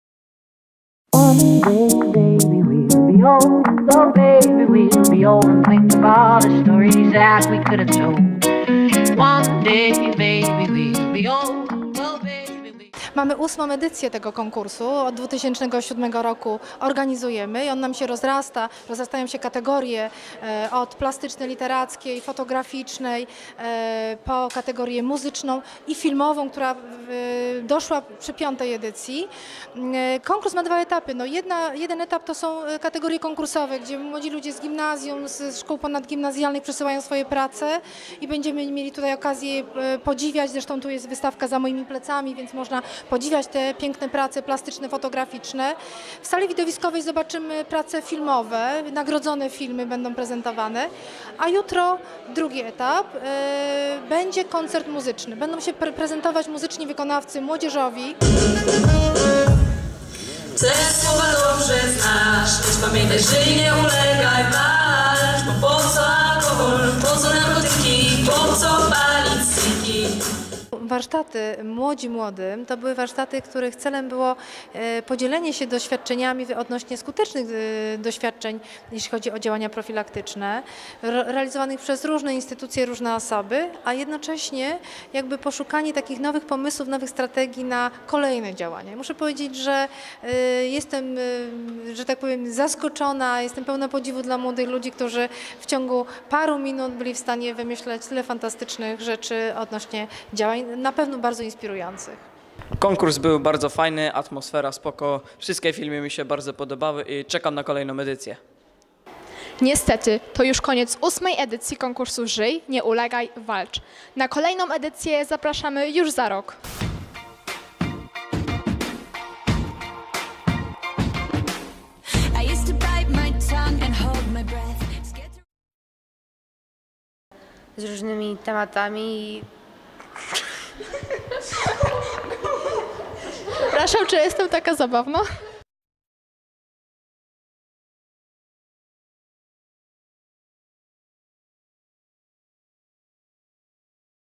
ŻYJ, NIE ULEGAJ, WALCZ.....!
Krótka relacja z konkursu Żyj, Nie ulegaj, Walcz...! Gimtv2 miało przyjemność brać udział w konkursie filmowym - już wkrótce opublikujemy nasze prace konkursowe.